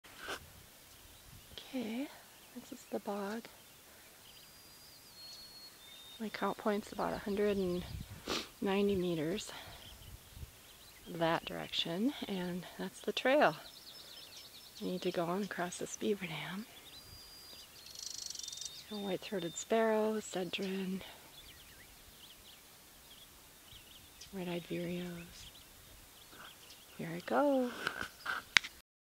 Agassiz point counts-beaver dam
Early morning at Agassiz, temp 48 F. I walked about 1 km from my vehicle to this beaver dam, which I had to cross to get into my bird survey starting point the bog area 190 meters away.